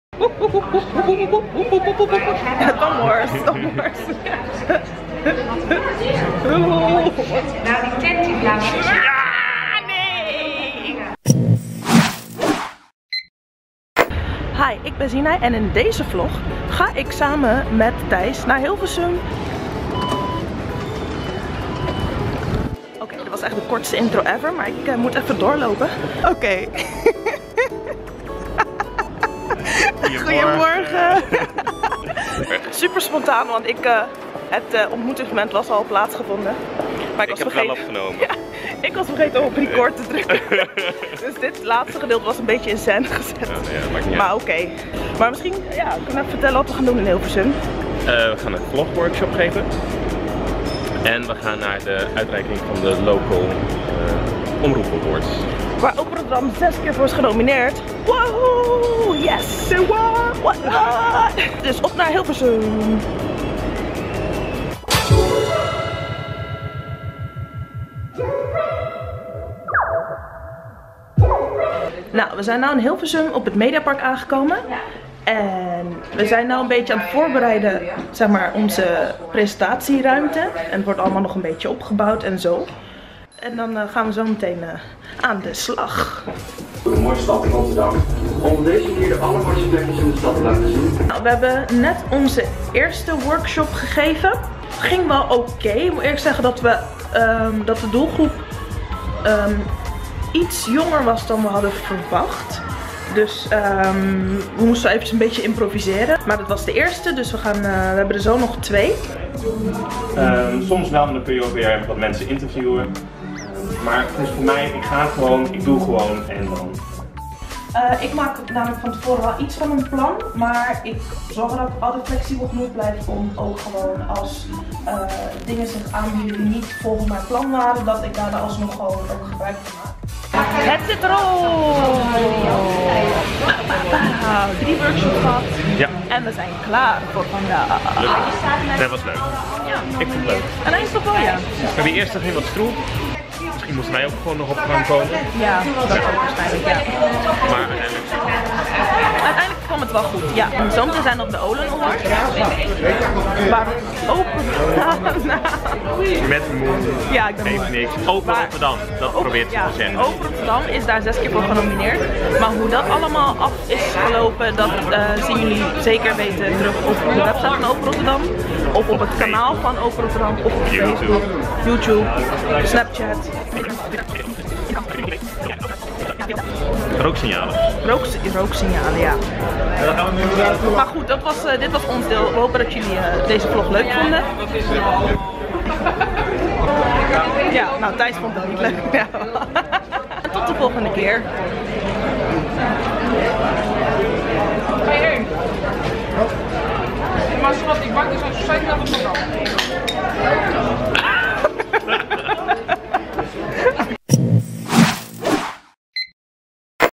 OPEN Vloggers Zeny en Thijs geven vlogles in Mediapark Hilversum | VLOG #106
OPEN Vloggers Zeny en Thijs gaan naar het Mediapark in Hilversum om tijdens de Open Studio Dagen workshops over vloggen te geven, als vertegenwoordigers van OPEN Rotterdam.